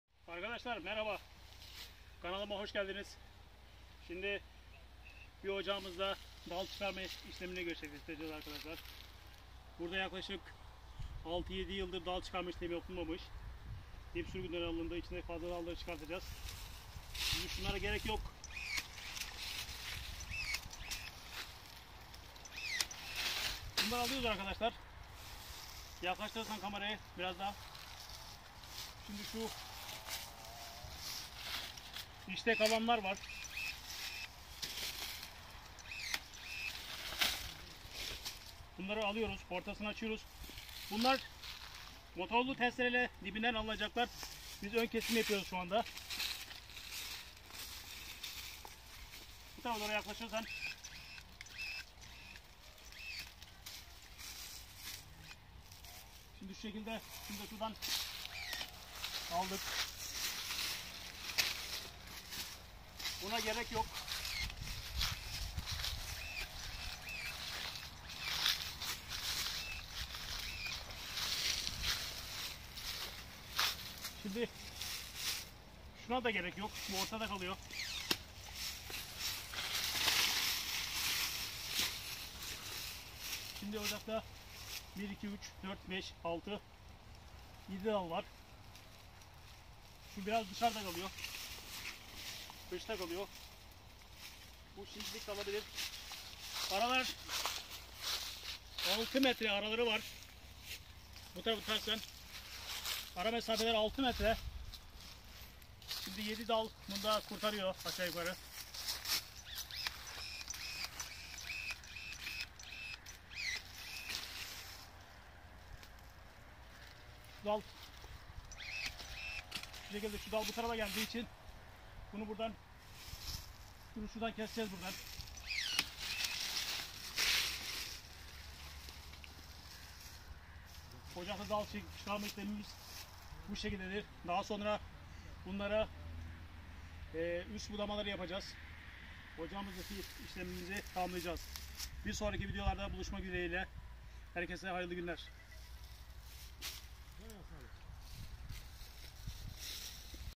FINDIK DAL ÇIKARMA
yaklaşık 6 7 yıldır hiçbir işlem görmemiş fındık bahçesinde dal çıkarma işlemi yapıyoruz